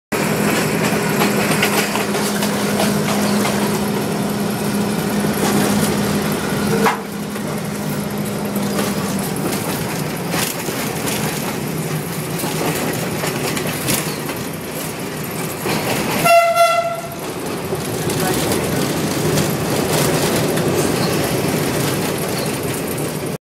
SLR Class M9 869 hulng the Kandy-Colombo Express train back to the parking-yard
Taken: At Maradana , Sri Lanka
Train : Kandy - Colombo Intercity Express Train
Locomotive : Class M9 869